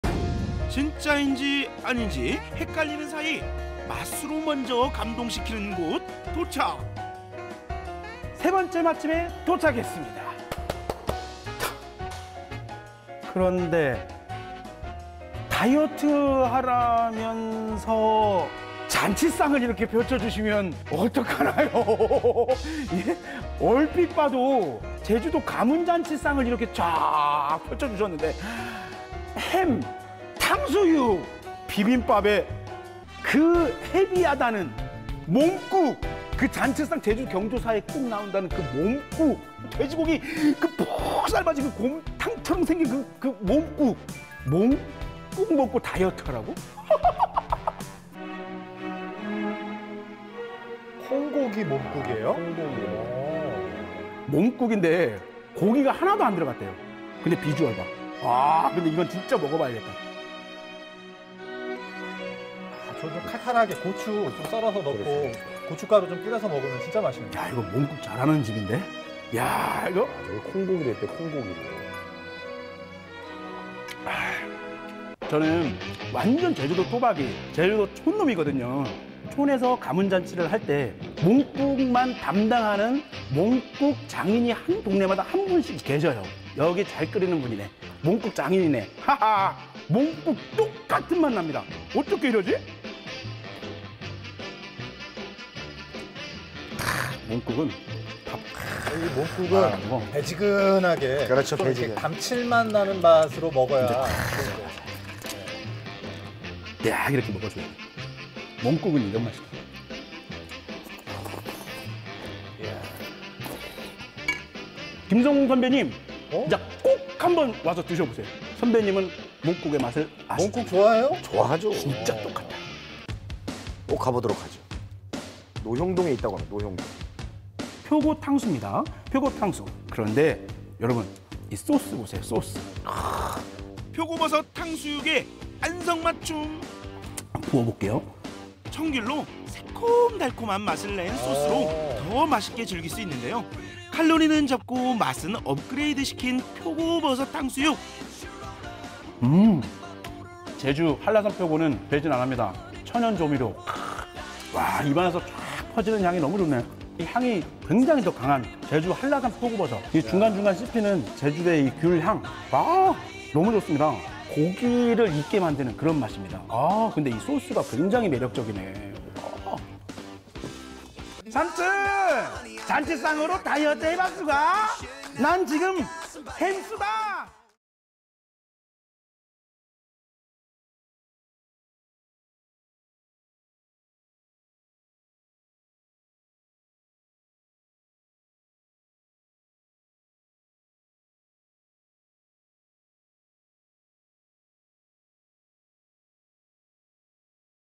콩고기 몸국과 표고버섯 탕수! 채식 다이어트 맛집 제주 '푸른 솔 맑은 향'
푸른솔맑은향

영업시간 : 매일 11:00 - 20:00 ※화요일 휴무※
주소 : 제주 제주시 고사마루길 109